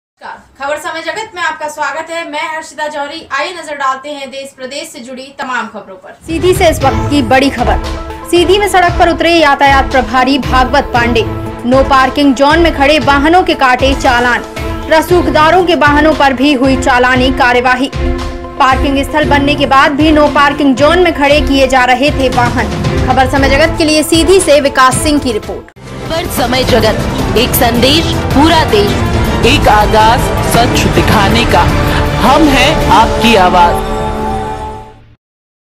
सीधी यातायात पुलिस की बड़ी करवाई, नो पार्किंग झोन में खड़े वाहनों पर की चालानी करवाई
सीधी से विकास सिंह की रिपोर्ट